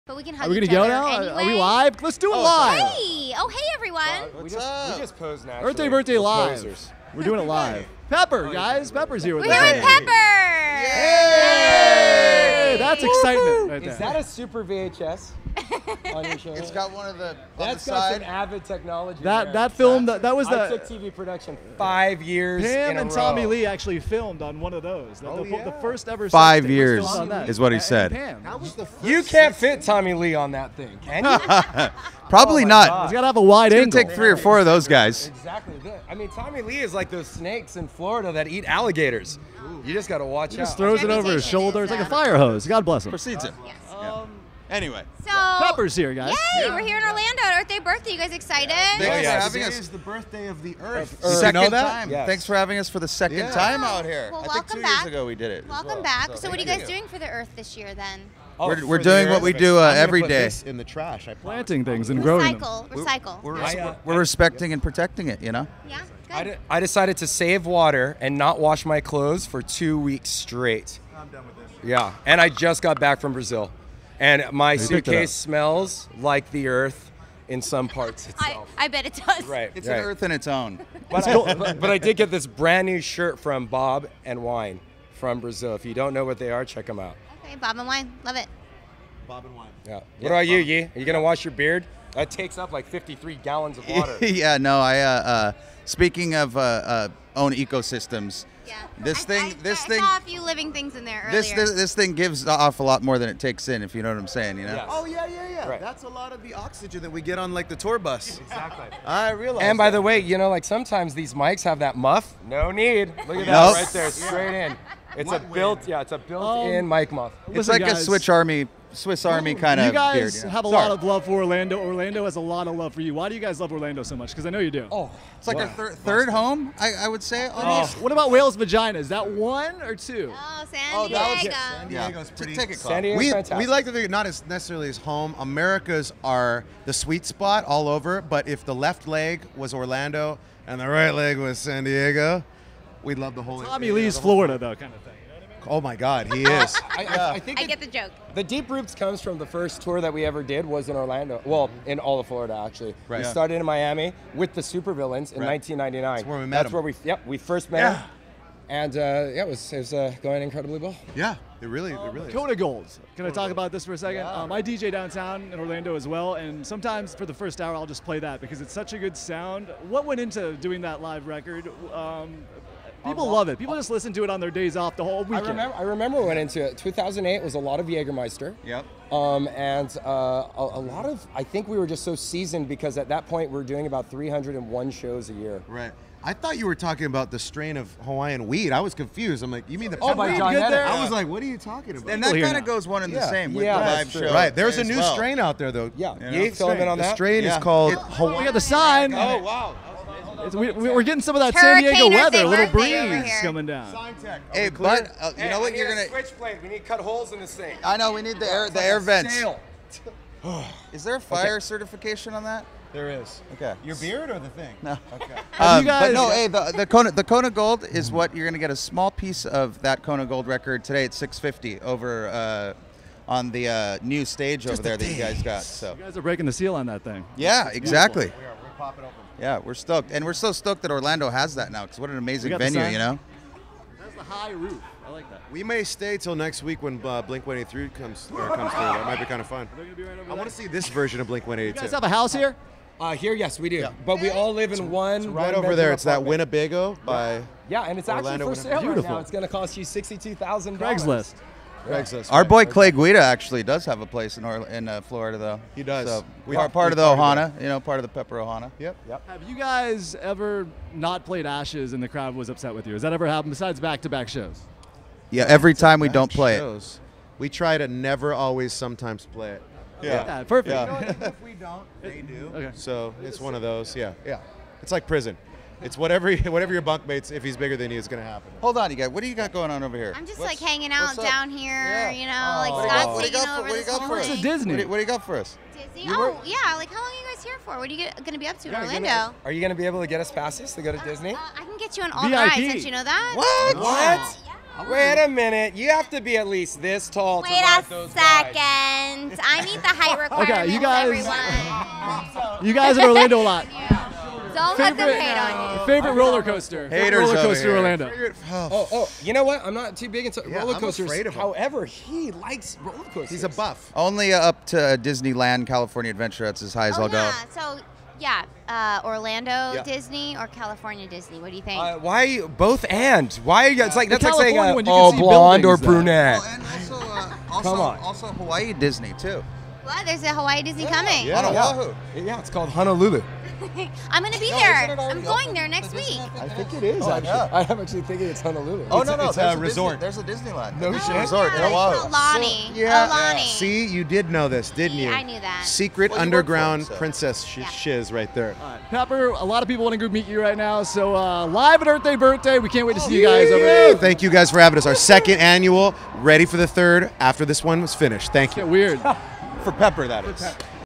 Pepper backstage at EDBD 24
Cori Yarckin and Scooter catch up with Pepper backstage at Earthday Birthday 24.